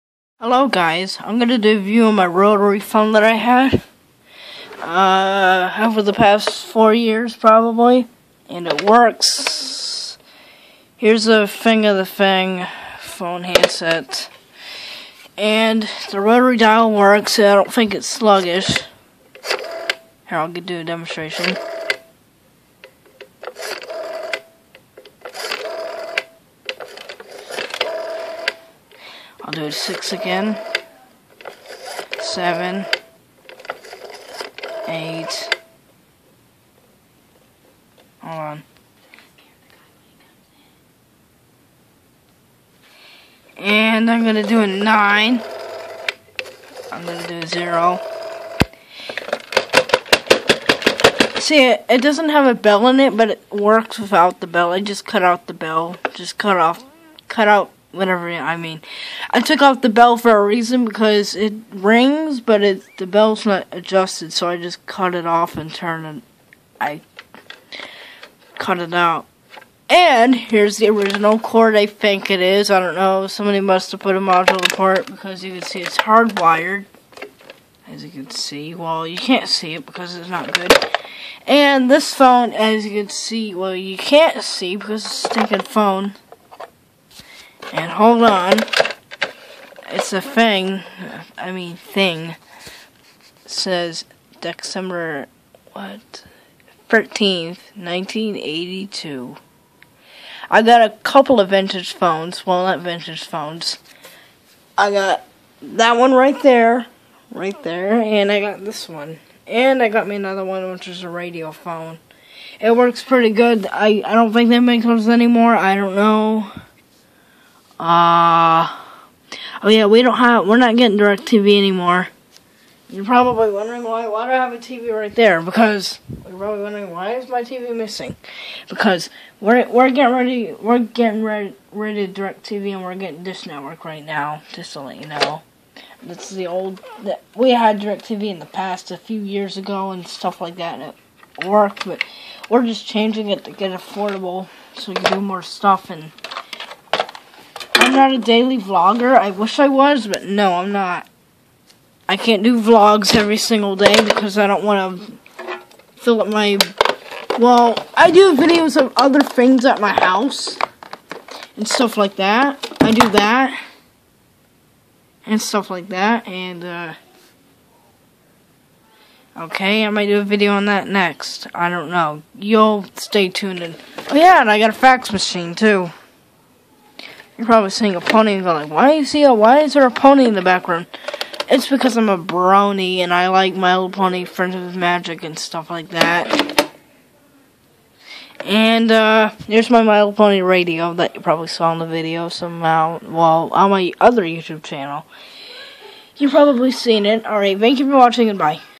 Rotary phone review
My rotary phone works I took up the pills for a reason cuz it ring right and the bells ring right because I don't know why I like the phone it works it works rotary works it dials out I love this phone I got it 1990